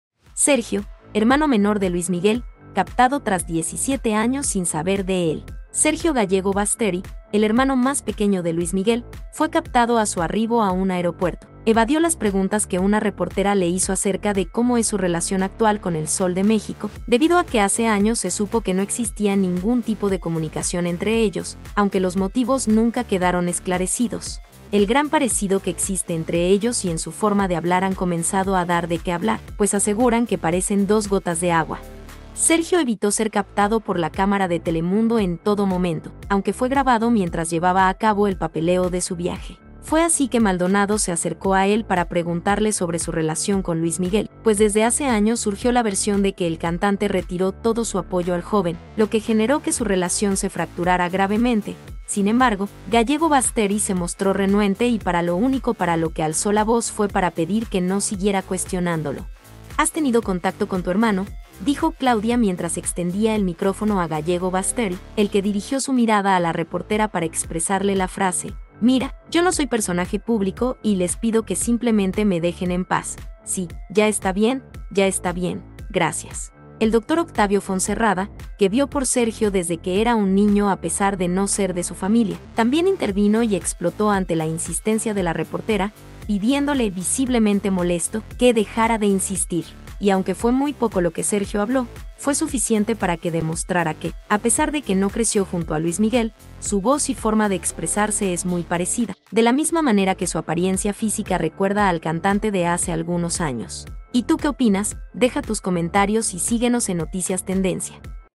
Sergio, hermano menor de Luis Miguel, captado tras 17 años sin saber de él
Sergio Gallego Basteri, el hermano más pequeño de Luis Miguel, fue captado a su arribo a un aeropuerto, evadió las preguntas que una reportera le hizo acerca de cómo es su relación actual con "el Sol de México".